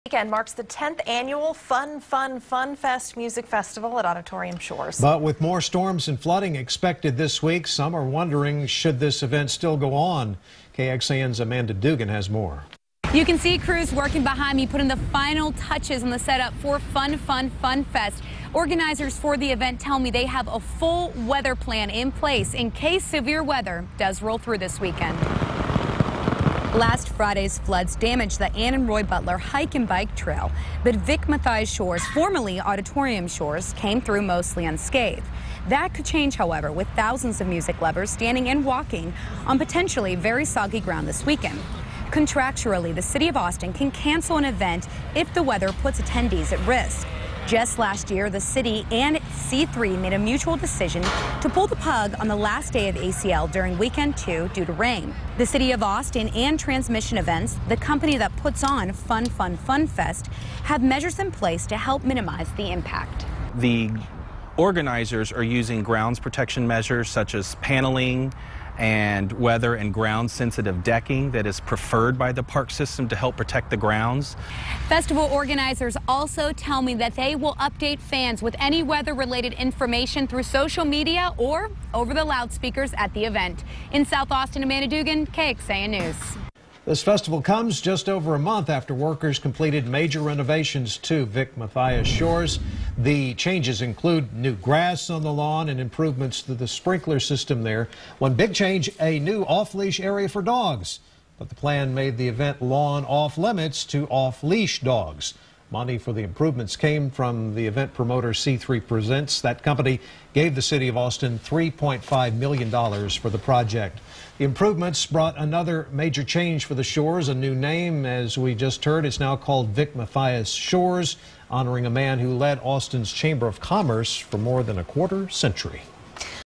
Fun Fun Fun Fest prepared for bad weather
Crews are making sure the grounds are prepped and ready for potentially soggy conditions.